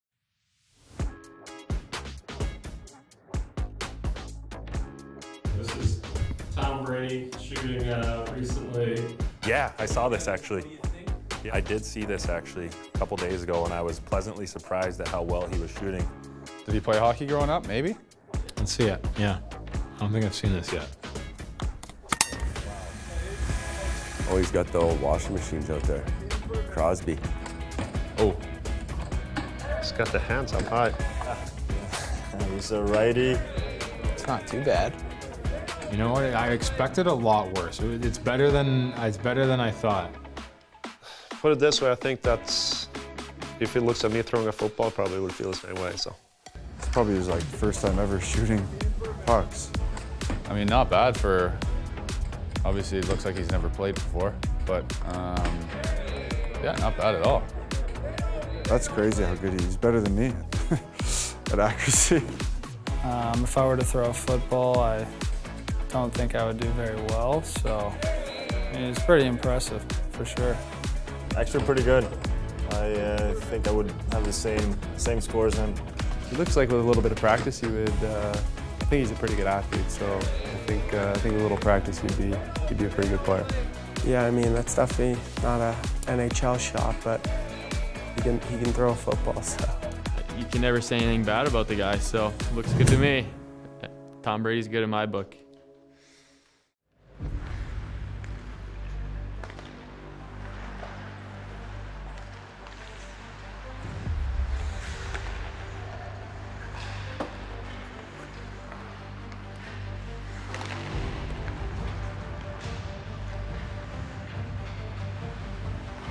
Rating Tom Brady's Hockey Shot | Puck Personality
Victor Hedman, Connor McDavid, Jack Hughes and Jack Eichel give their honest opinion on the G.O.A.T @tombrady hockey technique